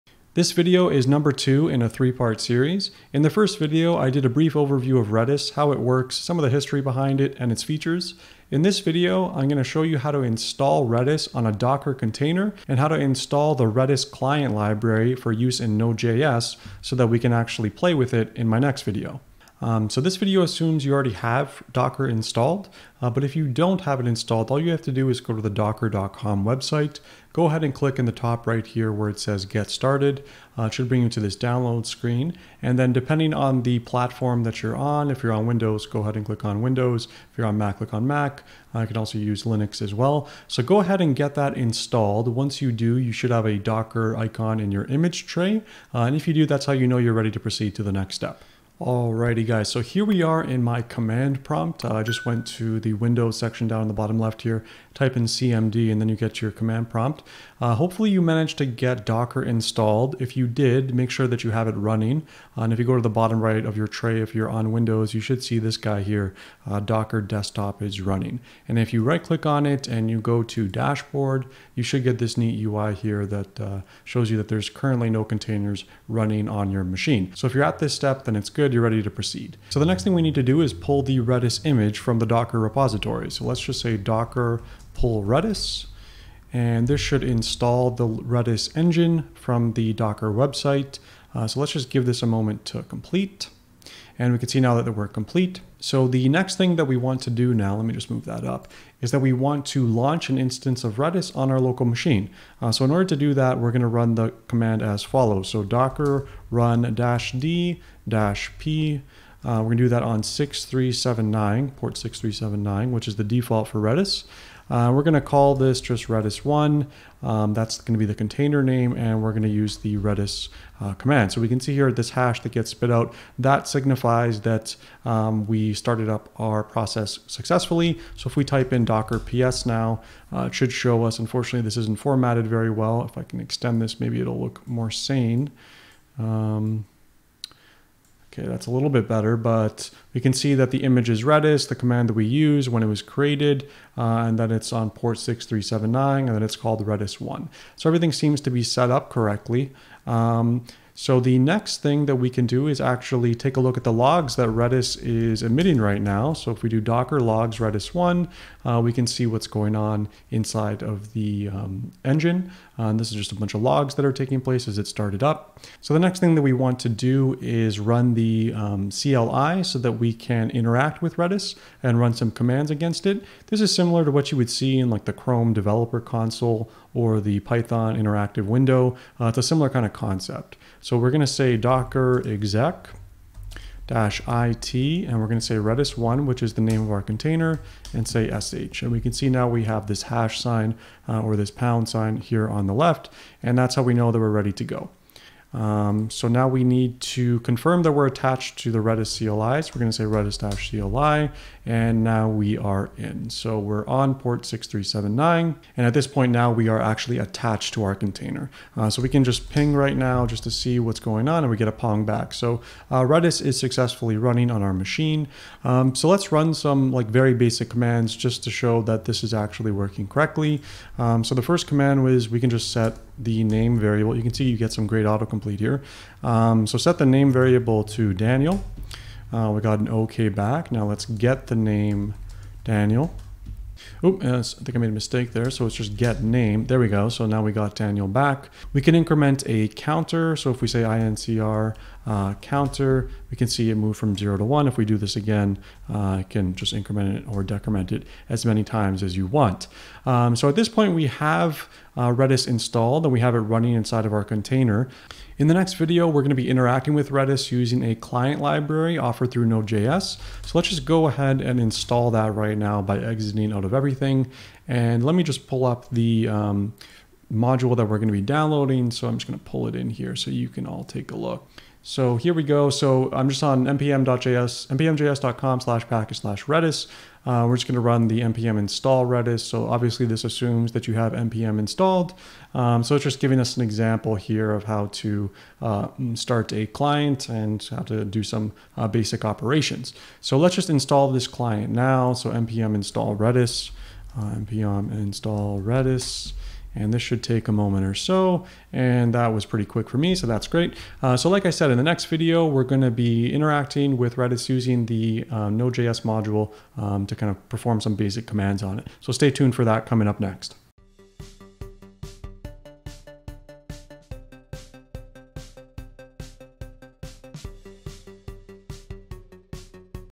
Learn how to Launch Redis on WINDOWS using DOCKER (REDIS CRASH COURSE PART 2 OF 3)
In this video, I show you how to install a local Redis instance on your windows machine using Docker!

Working Effectively with Legacy Code

☁Topics covered include:
Redis Windows
Redis Docker
Redis Image Windows